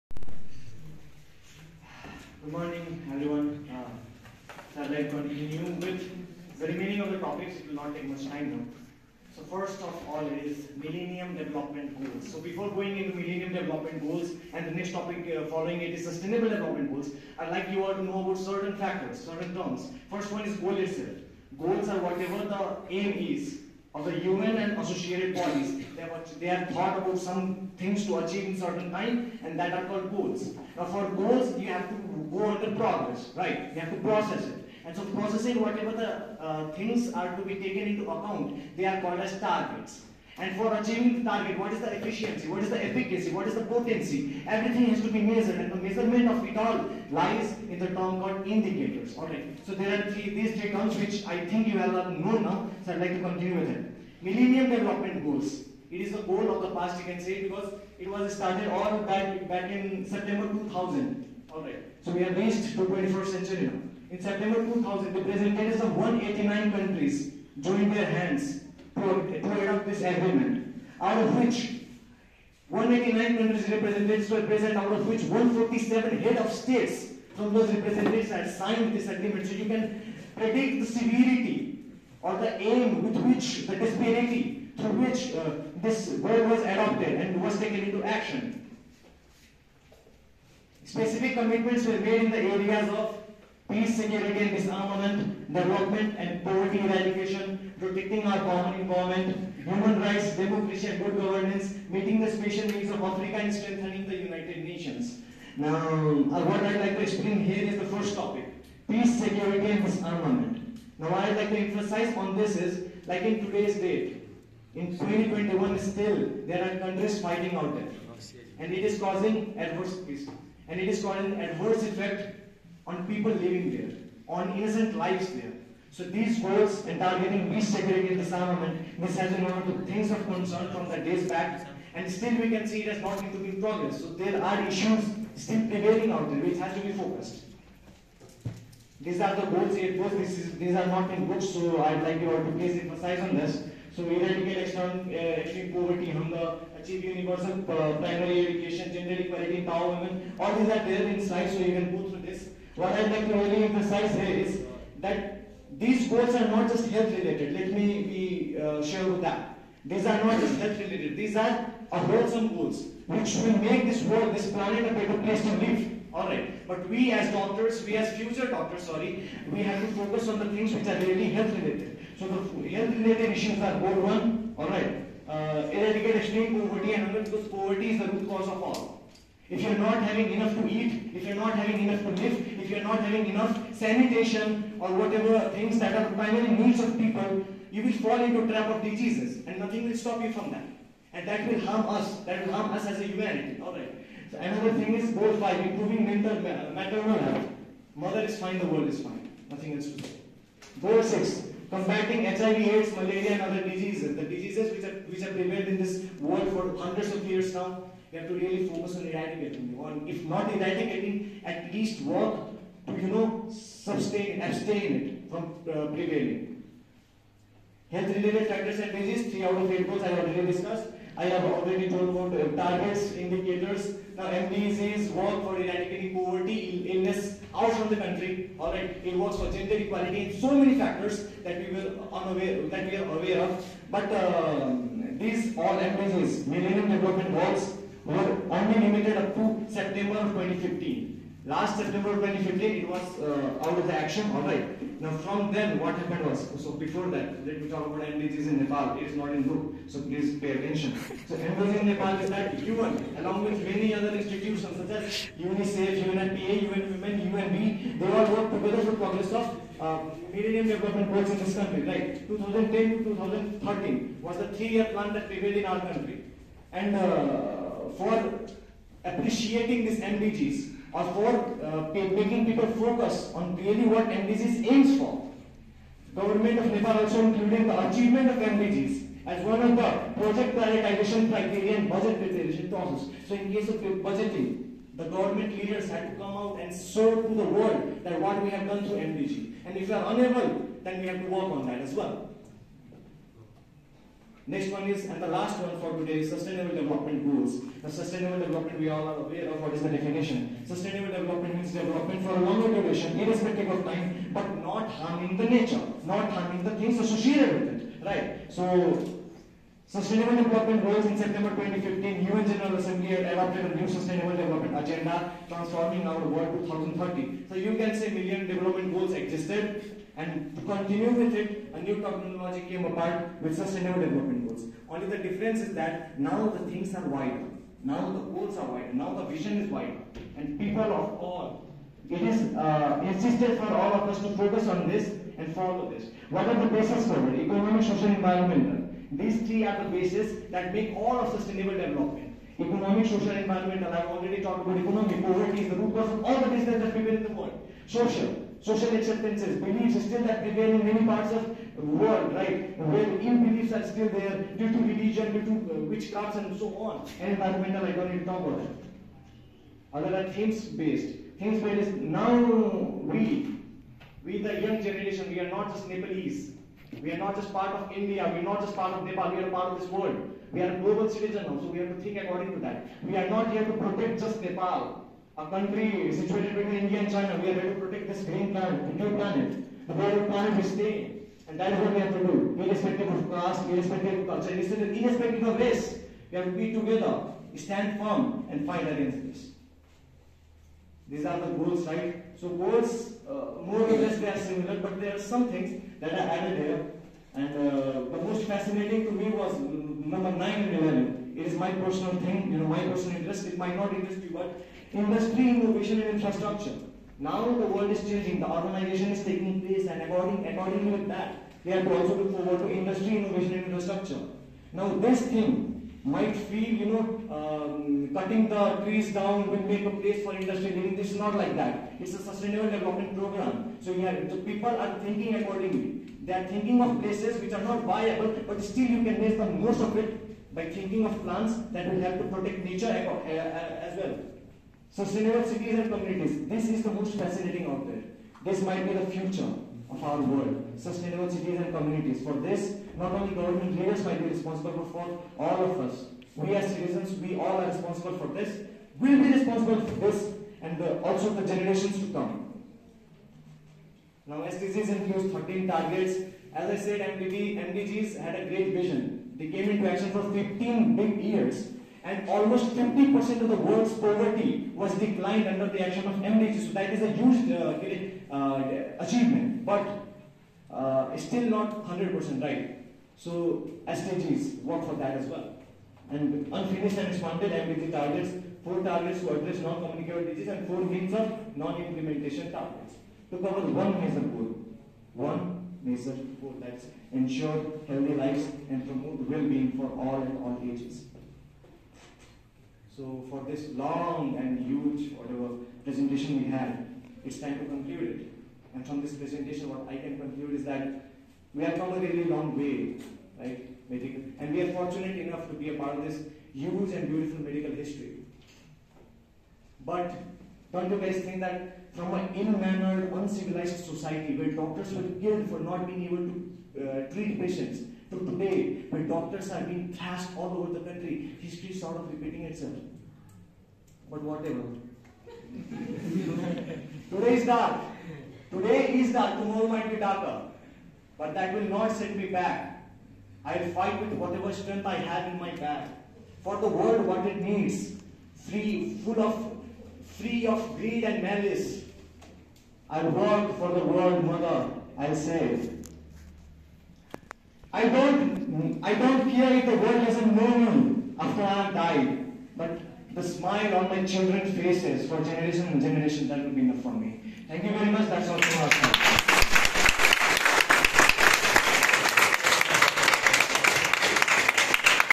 MDGs and SDGs by Manas Jha|| seminar MCOMS batch 2020!!
Manas Jha, first year medical student at Manipal College of medical sciences ( MCOMS)  presenting on Millennium Development Goals ( MDGs) and Sustainable Development Goals (SDGs) . It was the first seminar of batch 2020.